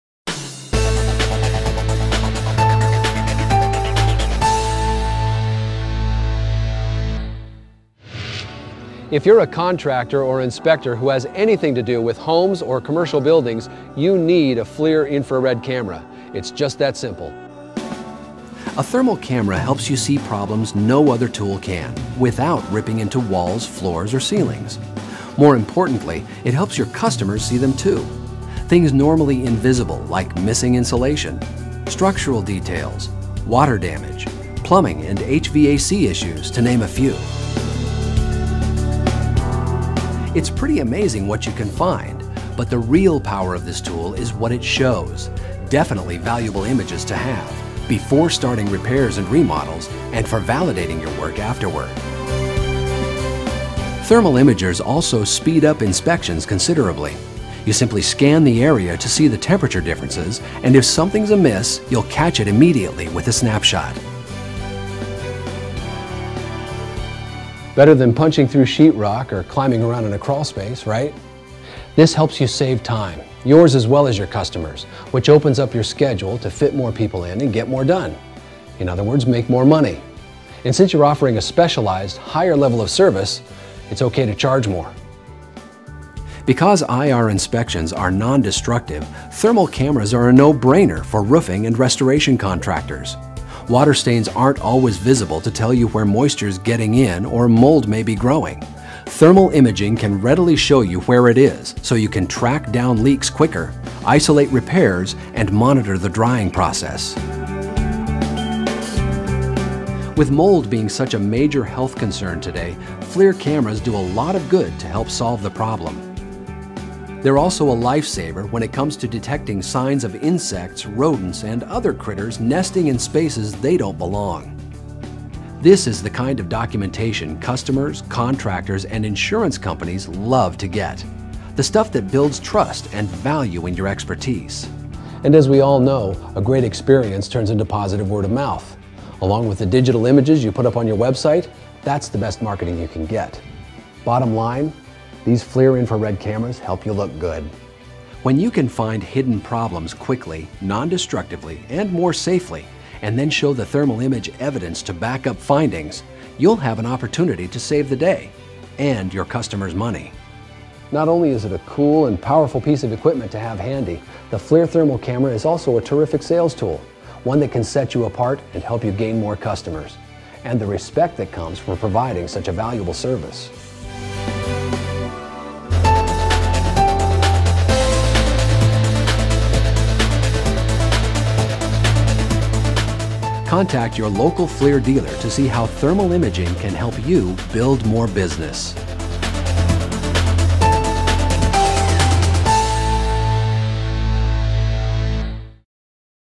Bygg-termografering med FLIR värmekamera
Informationsvideo om fördelarna med termografering med FLIR värmekameror vid bygg/vvs-applikationer.